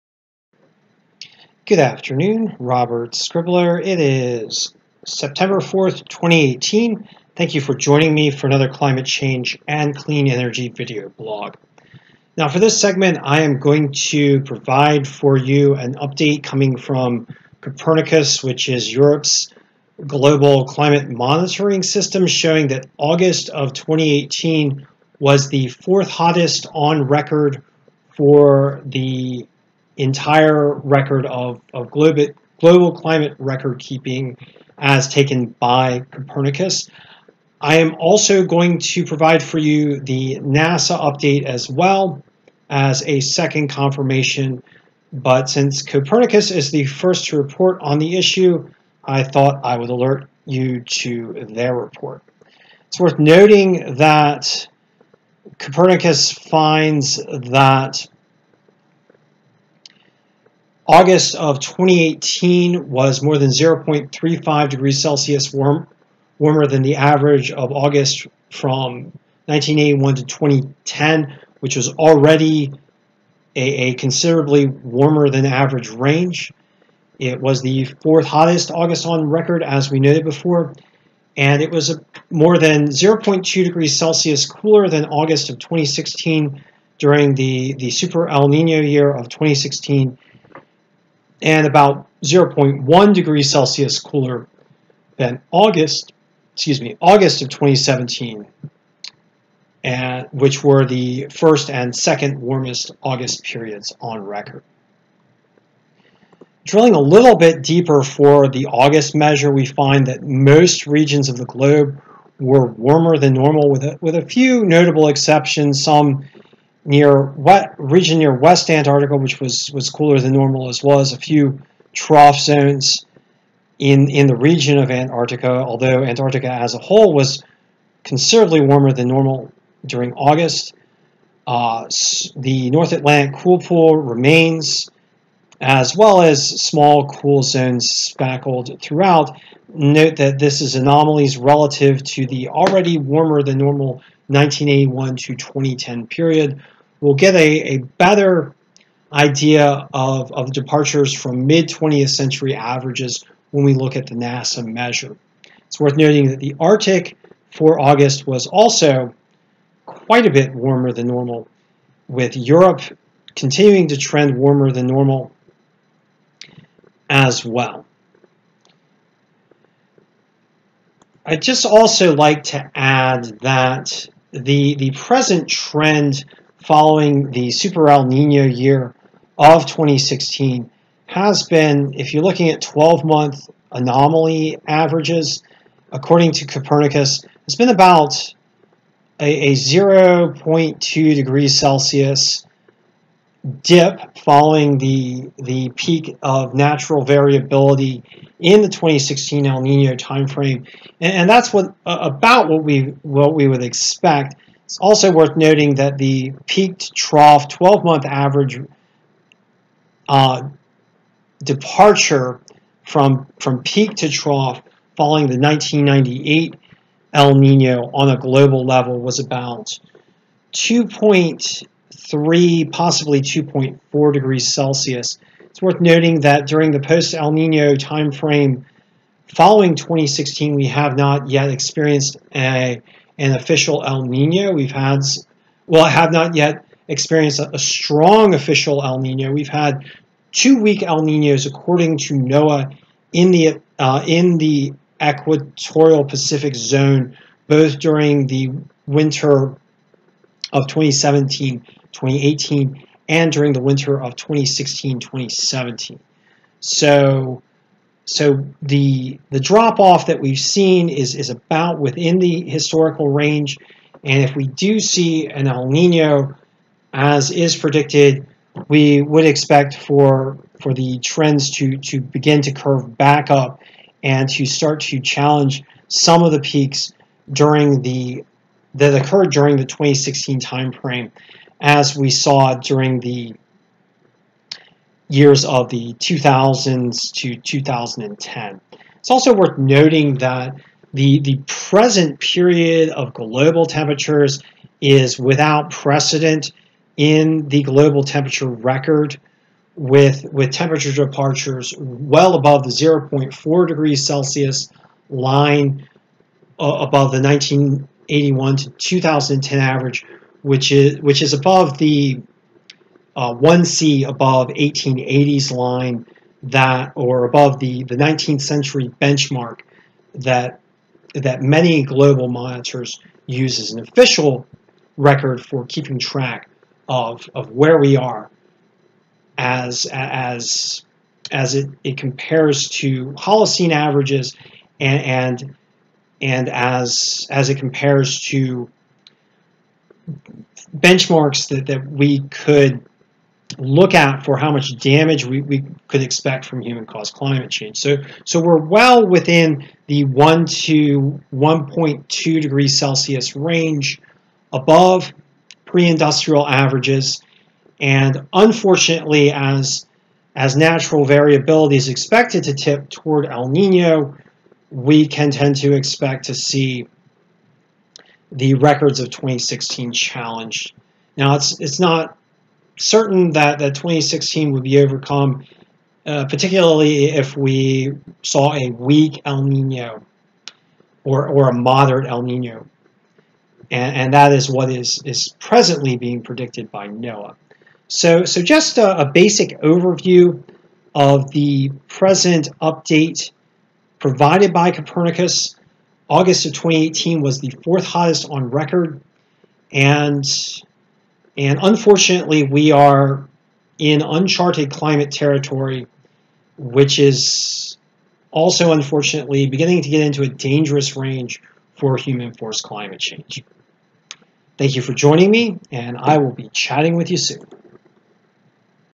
Copernicus: August of 2018 was 4th Hottest on Record
According to global climate monitoring by Europe's Copernicus, August of 2018 was the fourth hottest in the climate record.